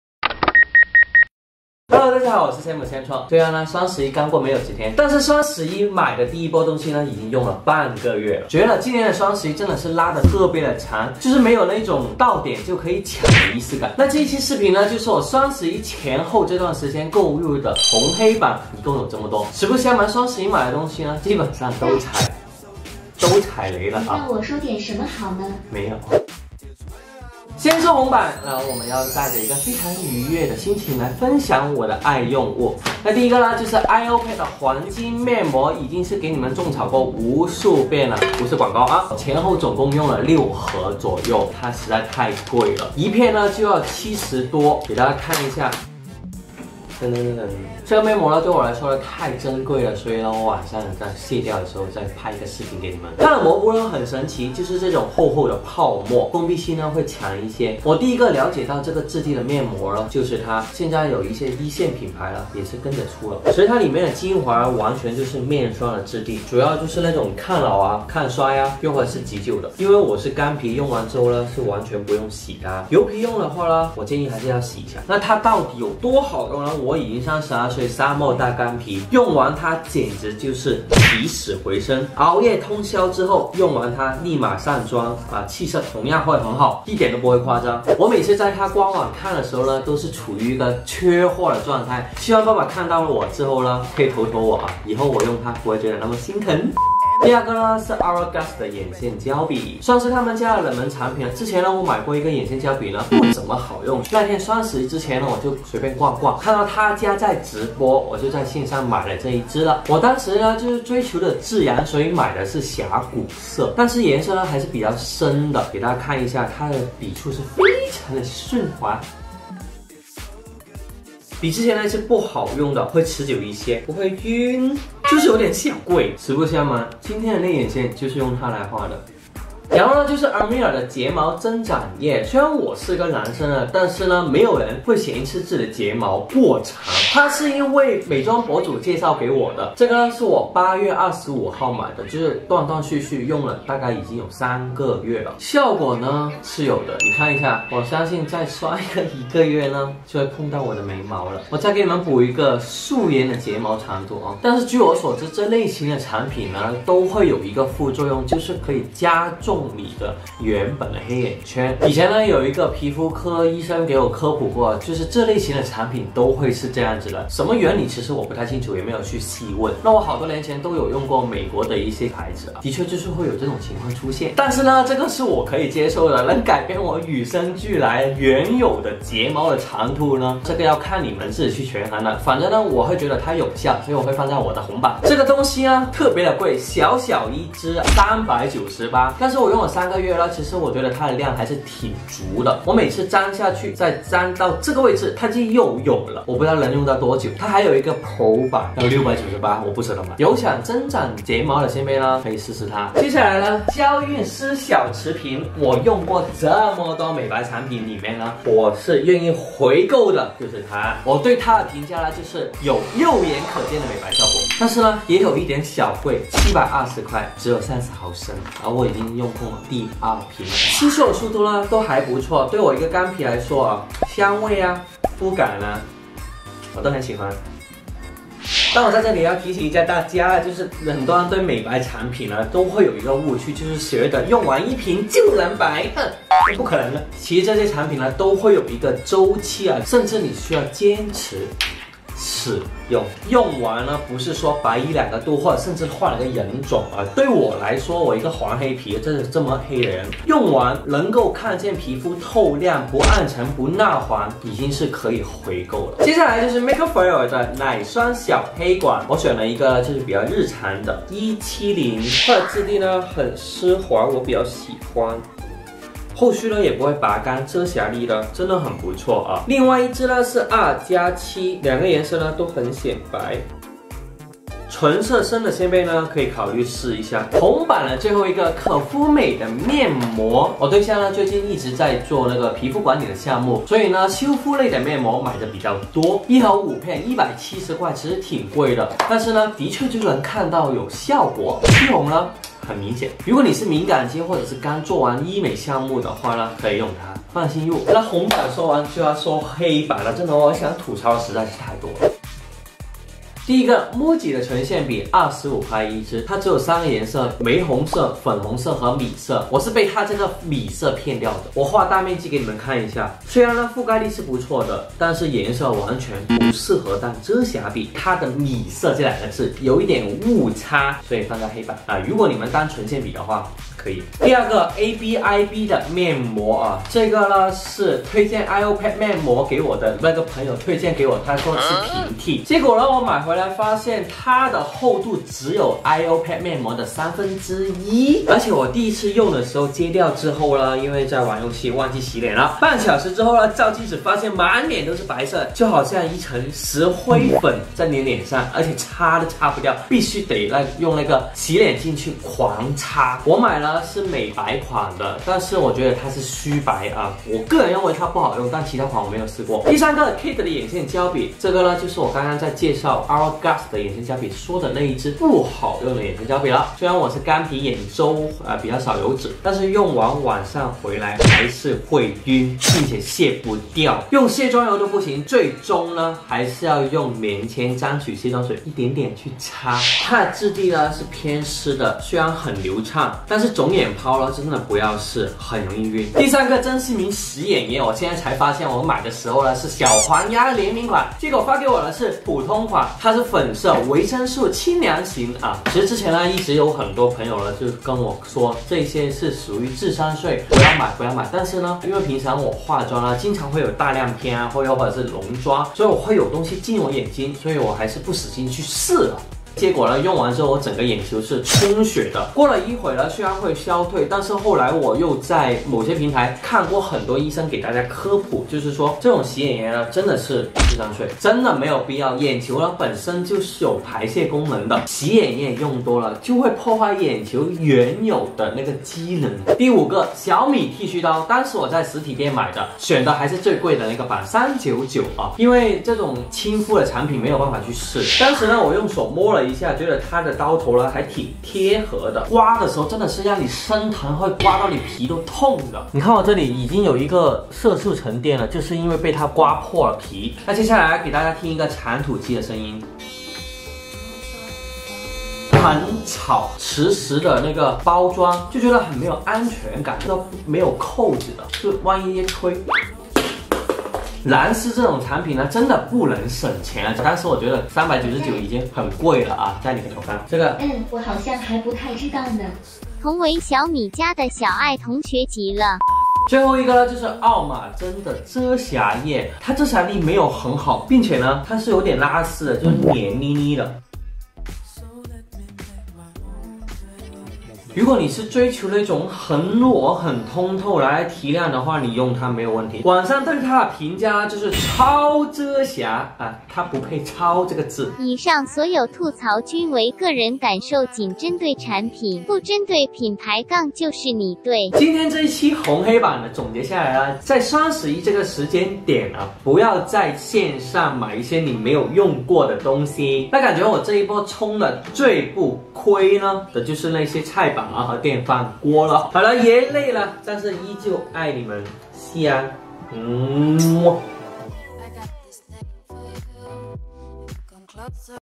《仙姆SamChak》仙试仙行｜双十一开箱红黑榜
【仙试仙行｜双十一开箱红黑榜】
红榜：让我容光焕发
黑榜：让我用的费劲巴拉
欢迎收看双11购物repo，又名「美妆购物半避雷指南」
亲身经验告诉你们，下单一时爽，垃圾火葬场

如果你喜欢这个视频的话，三连支持我吧！爱你们！